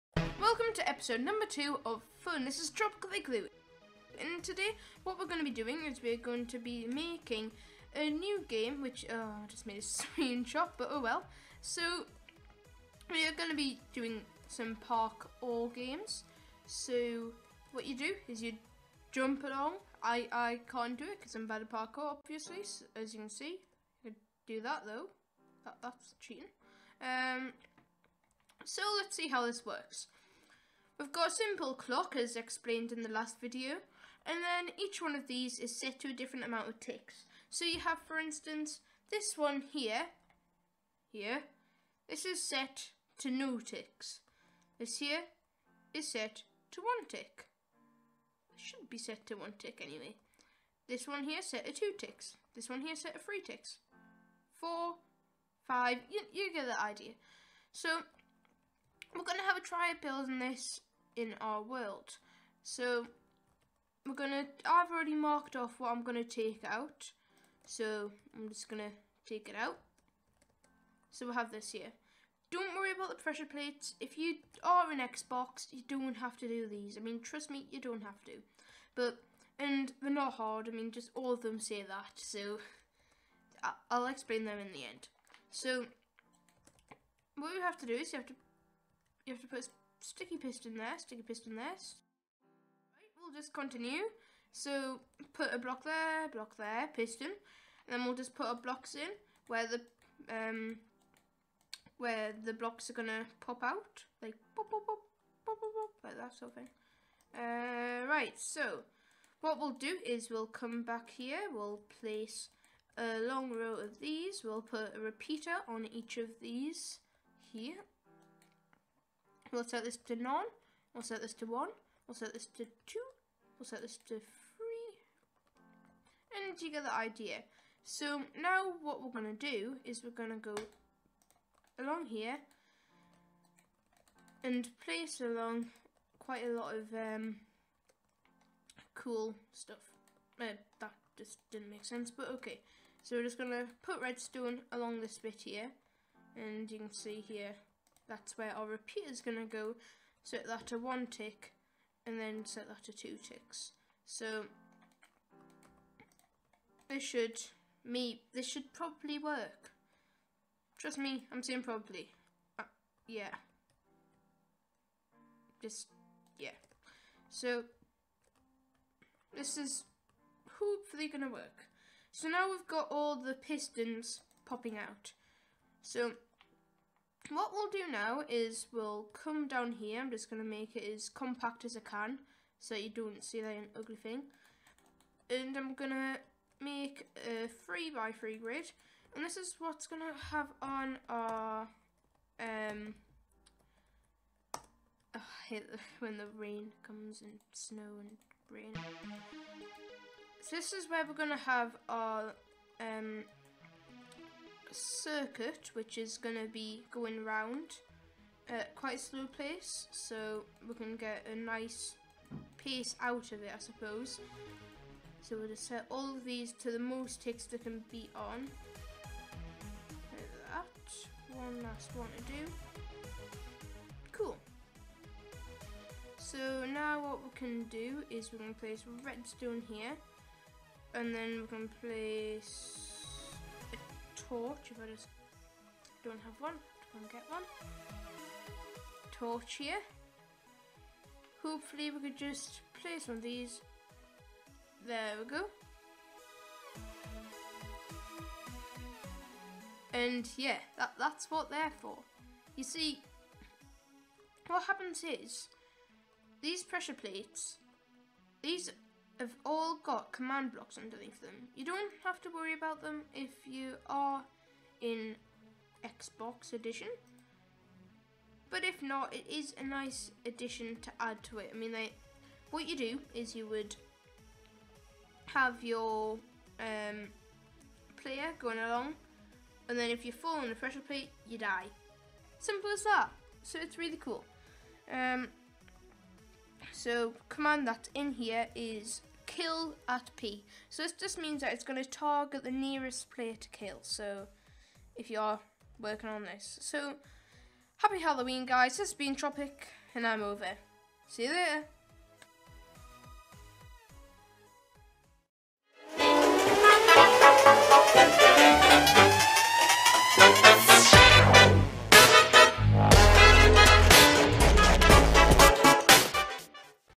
Creative Let's Play | Fun 1.0 | Parkour | E002
In todays video we make a part of parkour using pistons. I hope you enjoy